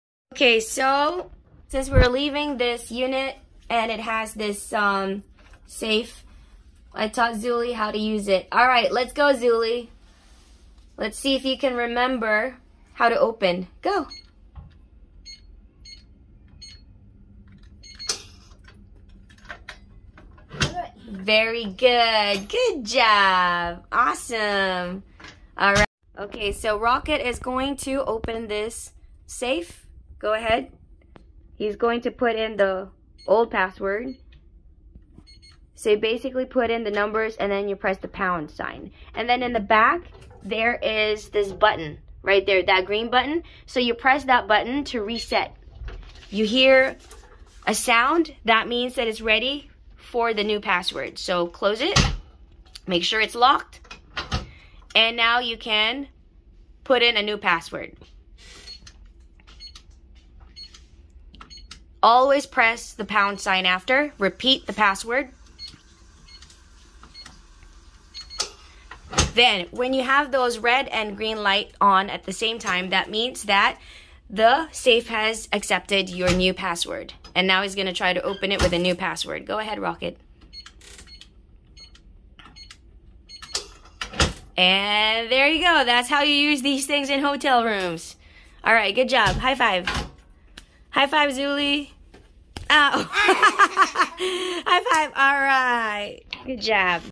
HOW TO Operate Hotel Safe. Easy Tutorial!
How many of us can't figure out how to work these things but are too shy to admit? After all these years, I finally learned. So I taught my 5 yr old and 19 yr old kids how to use it too! Hope you fibd this video useful.